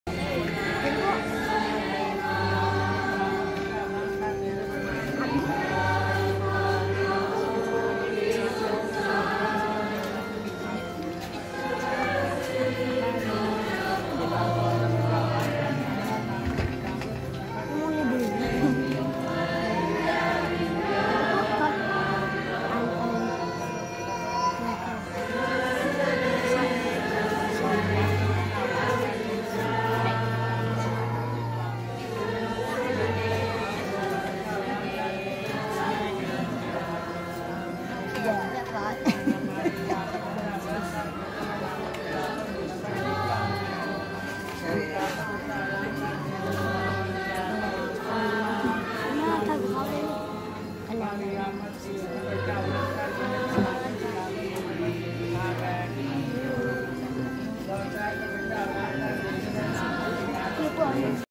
Bishop Panya Ketcharoen officiated the mass and the baptism ceremony
Winning souls; the Thai- Myanmar Border